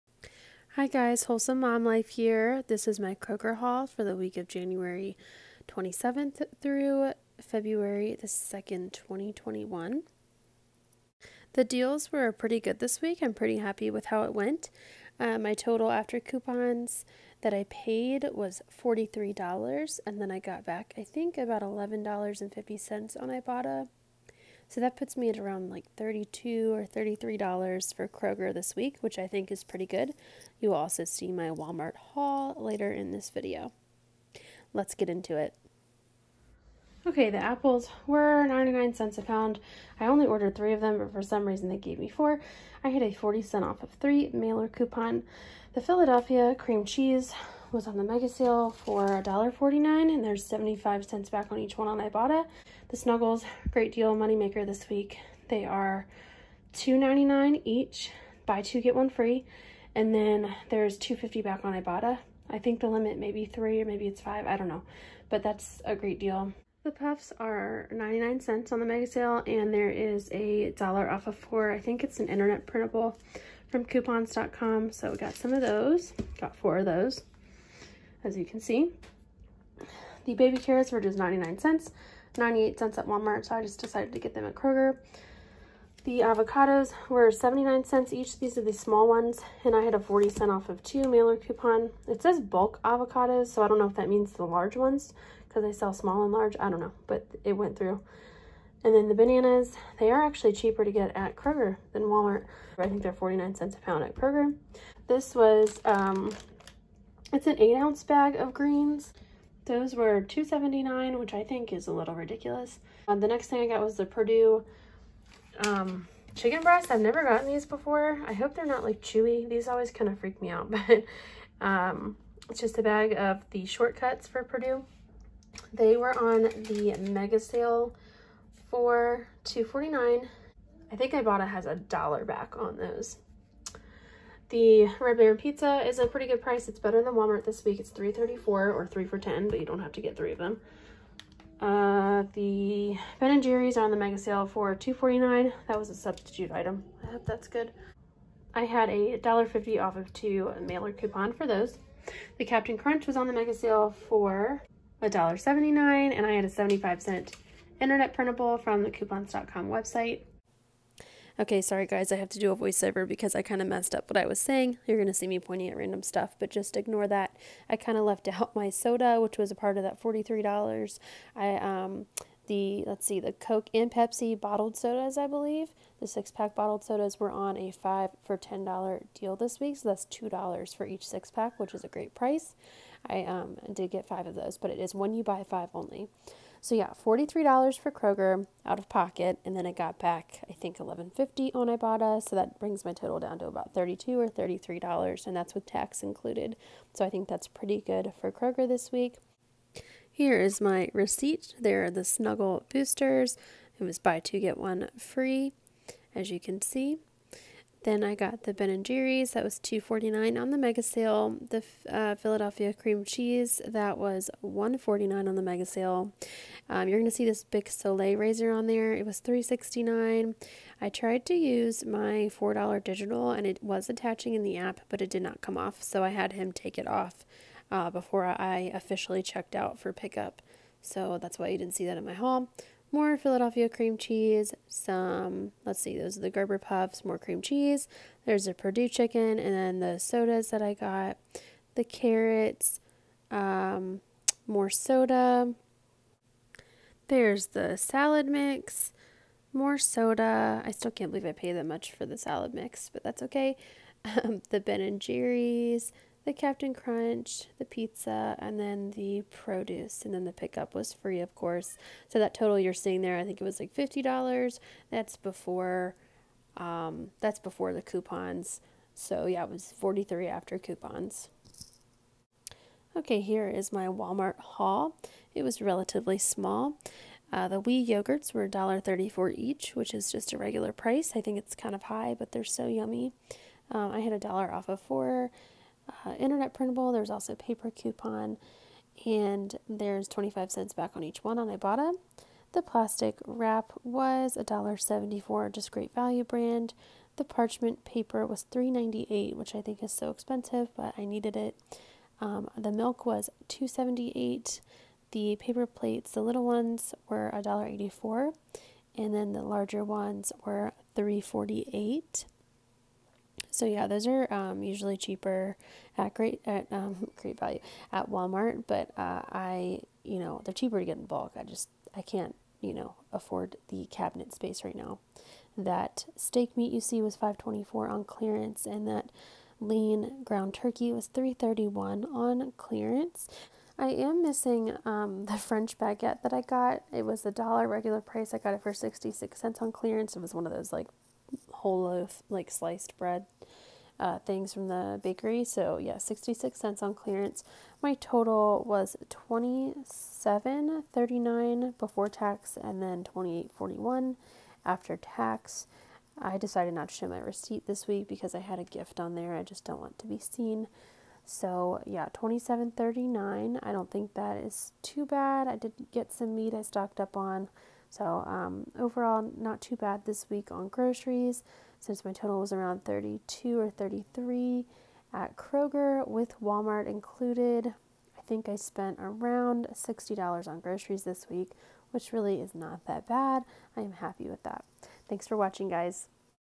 KROGER & WALMART 1/27/21 - 2/2/21 | Cheap Groceries! | Printable Breakdown
Hi there 👋. My name’s Olivia and I’m here to show you what extreme couponing really looks like on a weekly basis. Now let’s save some money! Happy couponing 😀

Ibotta code: gvixcwi
Fetch code: PRAMX